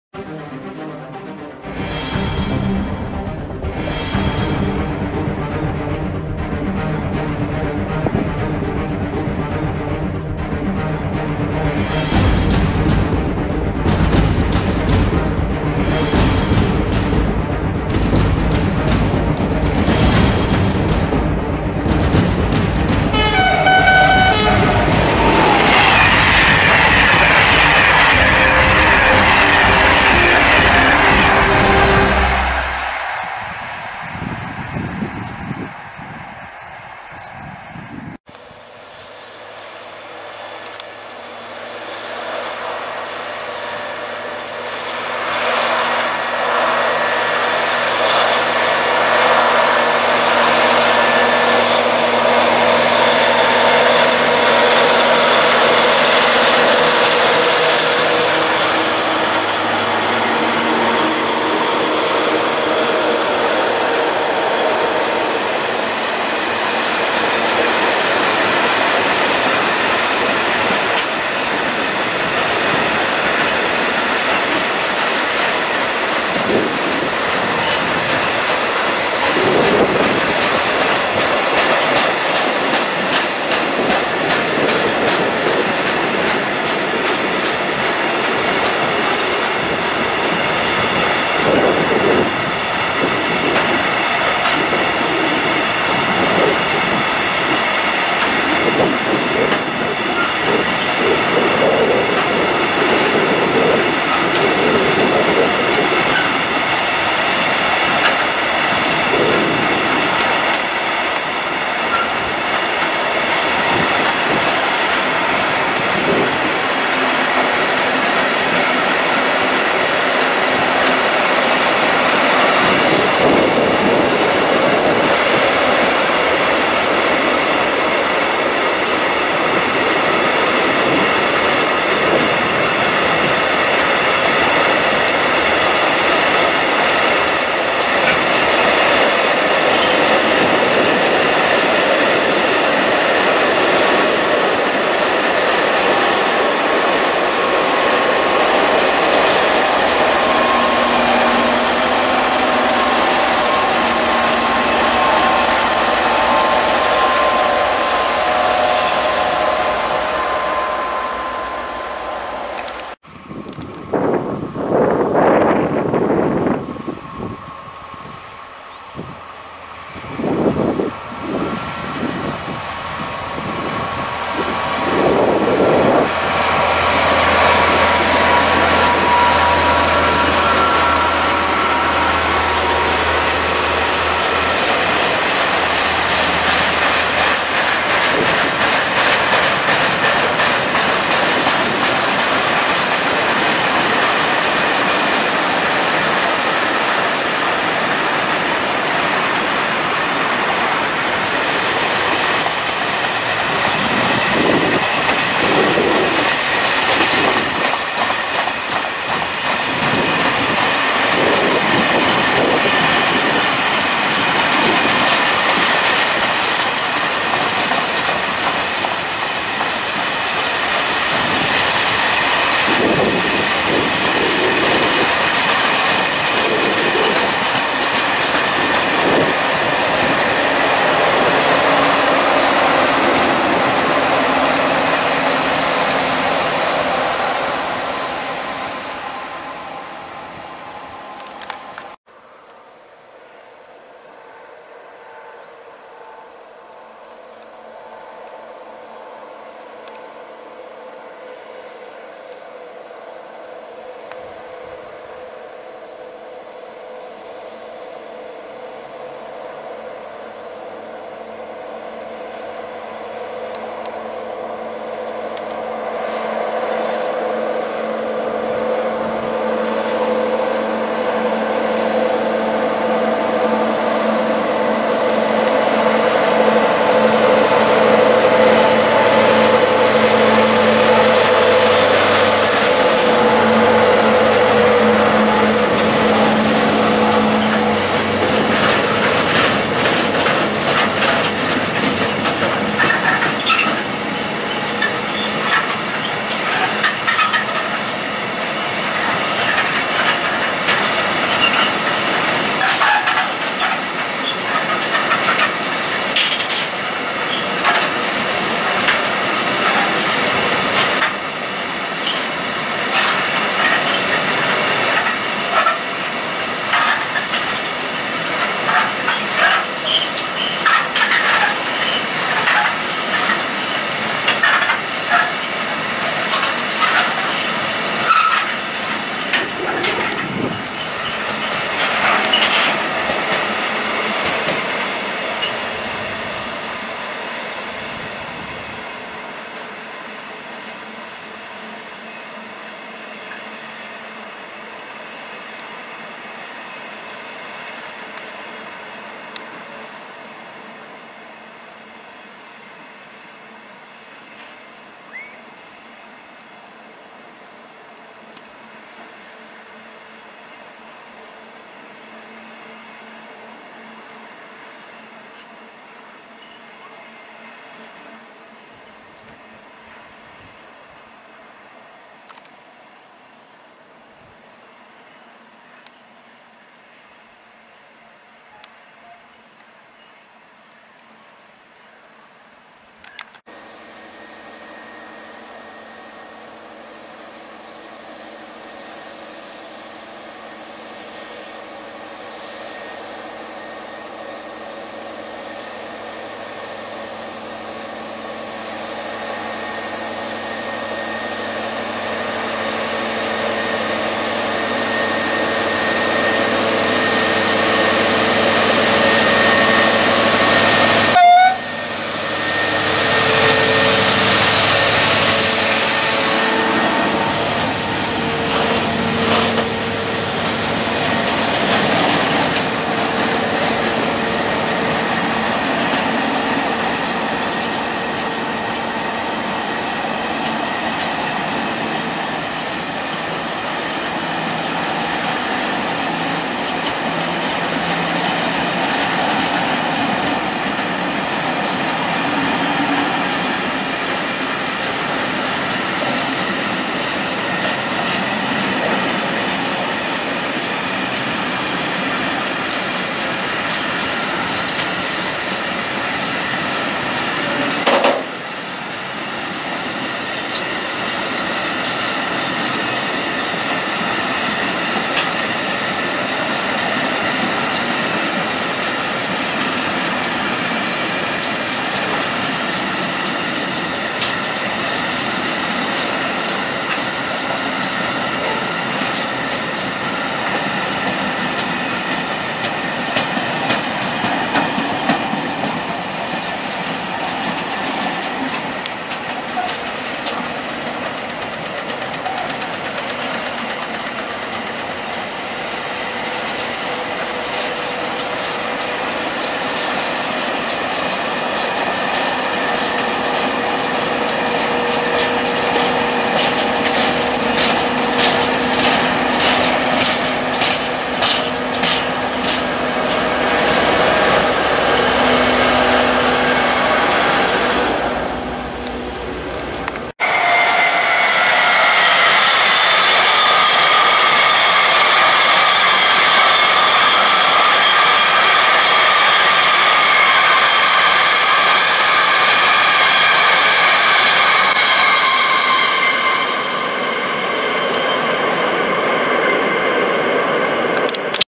retour a la base des ttx,28 aout 2020
fin de journée pour les TTX travaillant sur l'artére Nord/Est,passages a Dompierre/Helpes,sauf le dernier train, qui lui a été pris  a l'approche d'Aulnoye pour Tergnier.